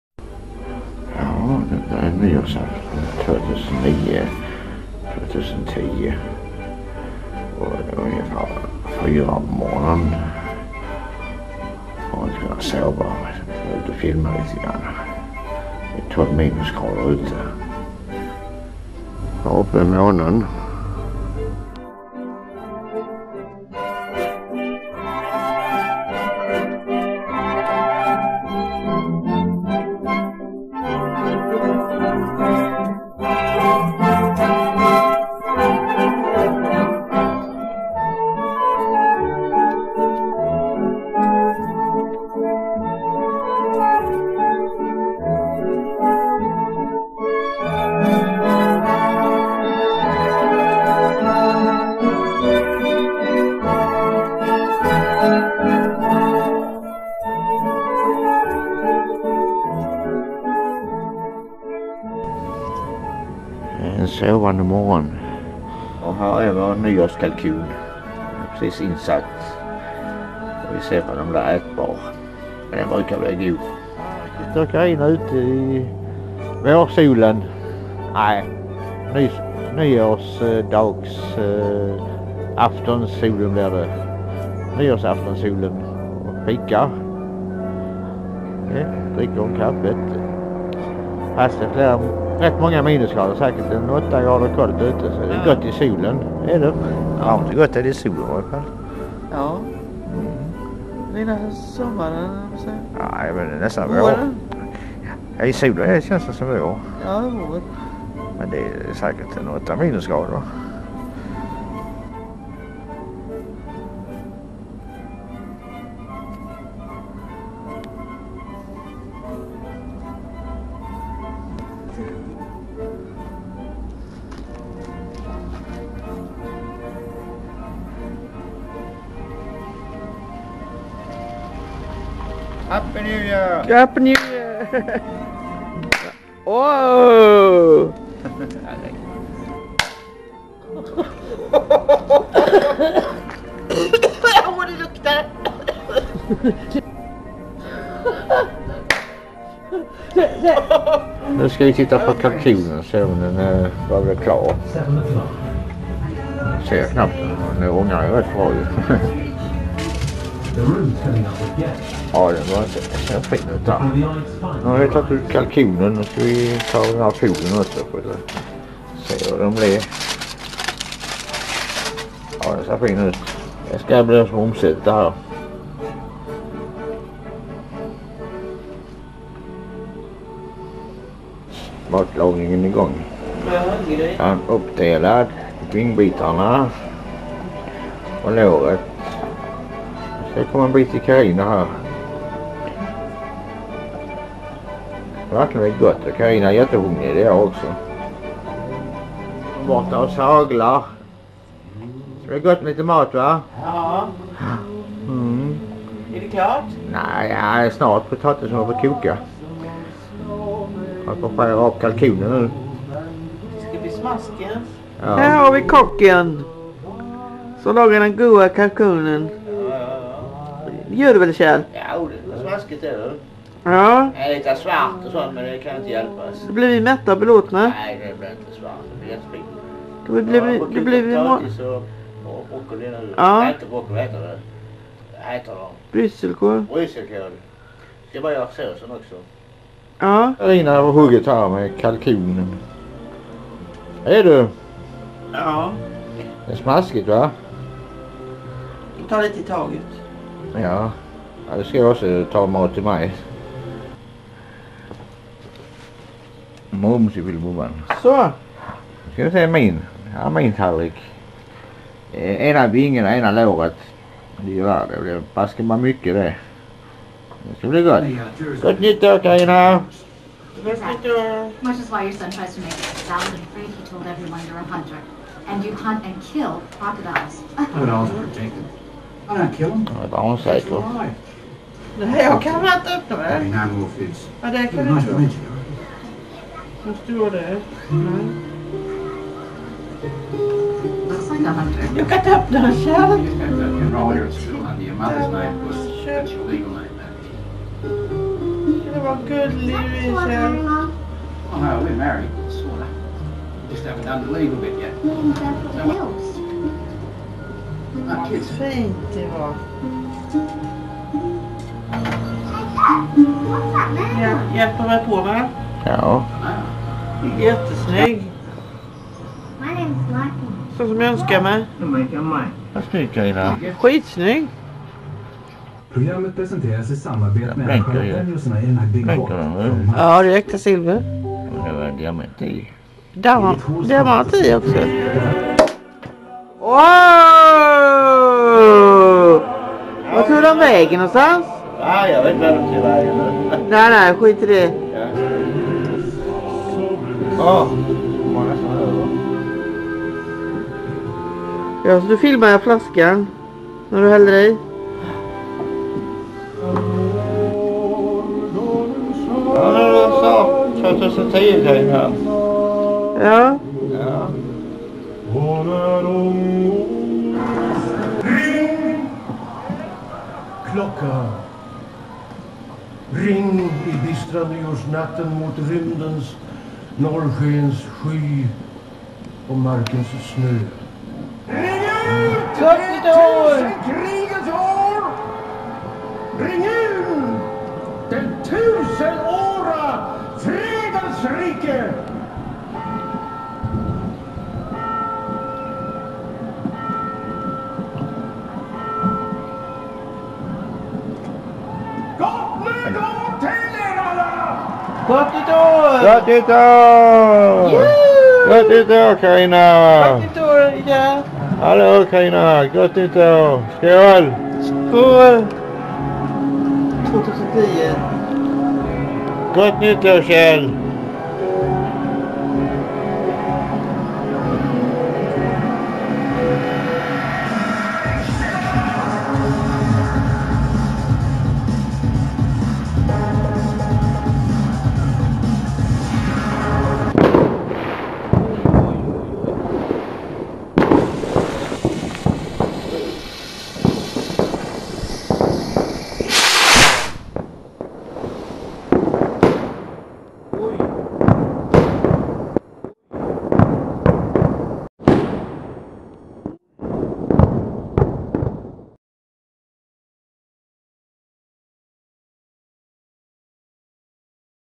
Nyår 2009-2010.mpg
Filmade vårt Nyår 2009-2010 i Varberg Sverige.Vi hade Kalkon sm Nyårsmat